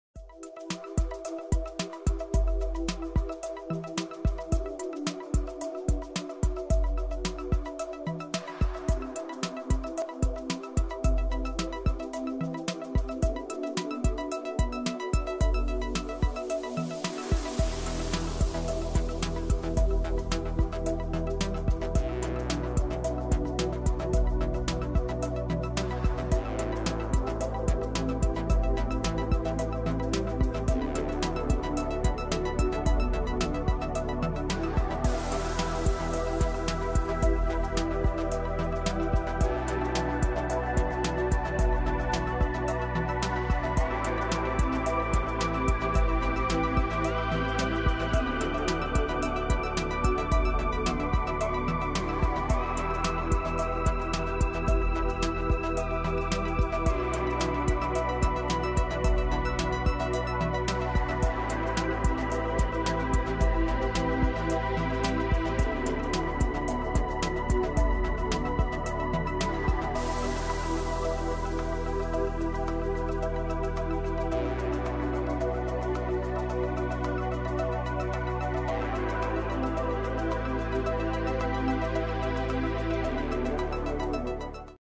Who is Rod Rosenstein?
A group of 11 Republican lawmakers are trying to impeach Deputy Attorney General Rod Rosenstein because of concerns about his oversight of the Department of Justice and its investigation into suspicions of collusion between the Trump 2016 campaign and Russia.